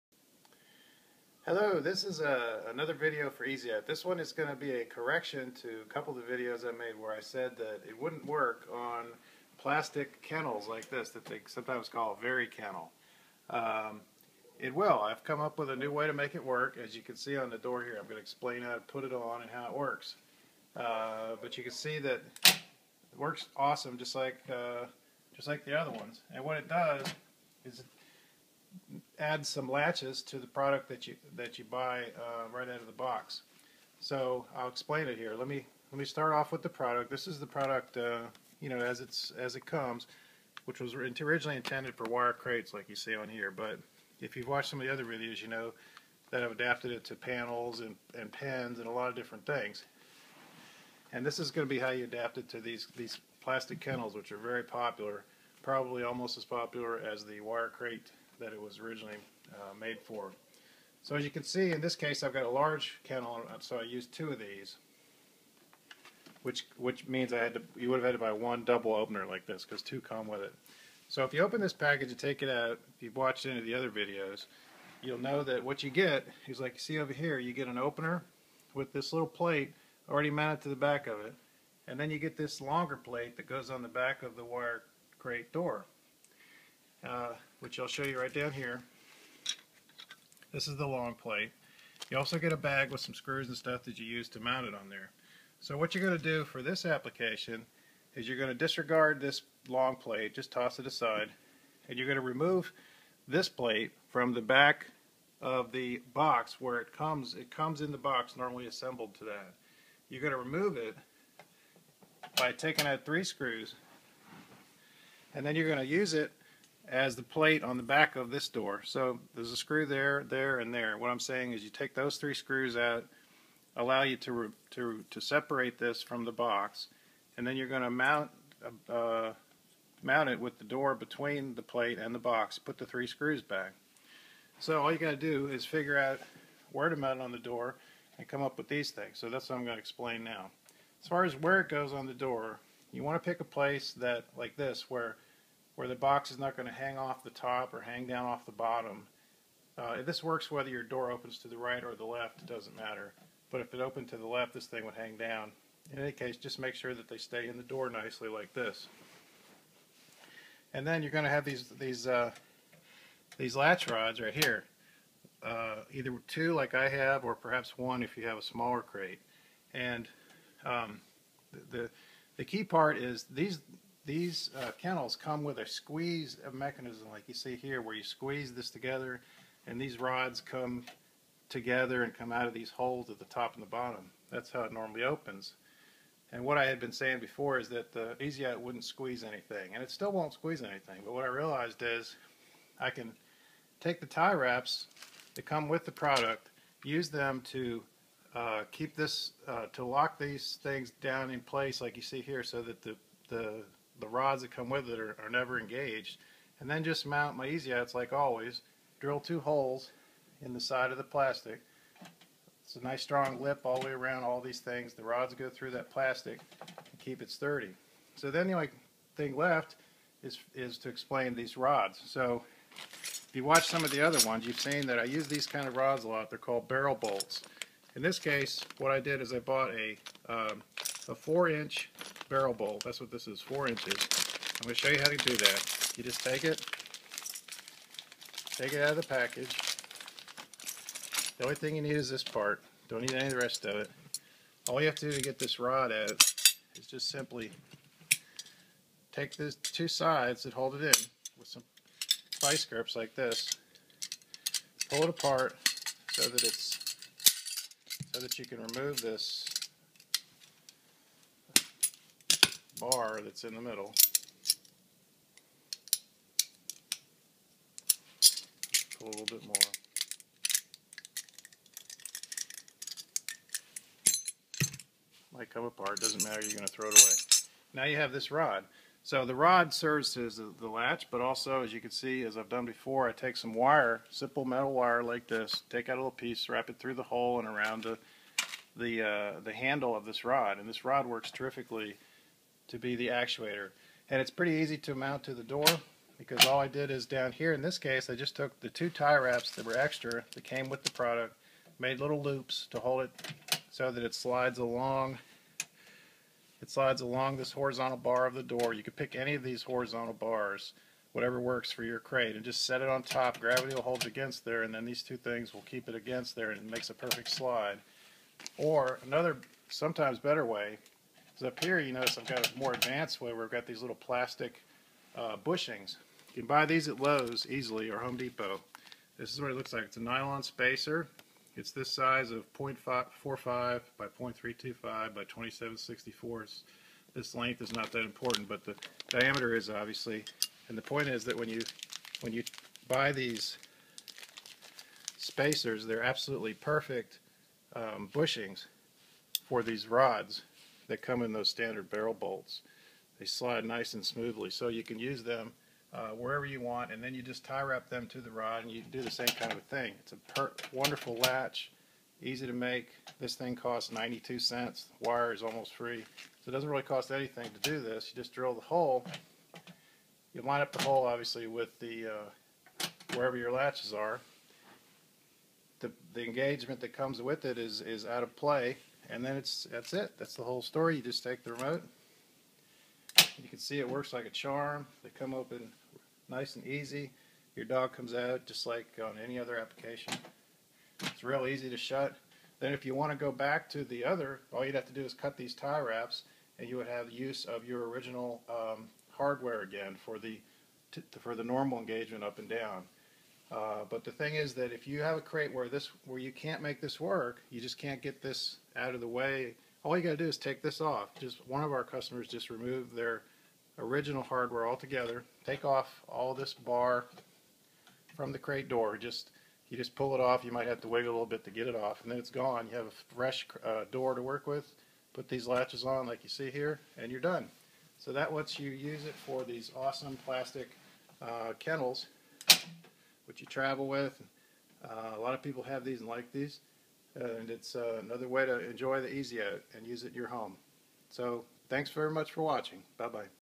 Easy Out video 01a - Introduction Supplement Adding Plastic Transport Carriers!
New and exciting news! This video explains how the Easy Out can be used on those very popular plastic transport carriers, sometimes known as "airplane style carriers" or "Vari-Kennels".  Some of the original Easy Out videos said the Easy Out would not work on these type carriers since they required a squeezing motion for their latches, but this video explains how to easily overcome that issue, allowing the Easy Out to work great!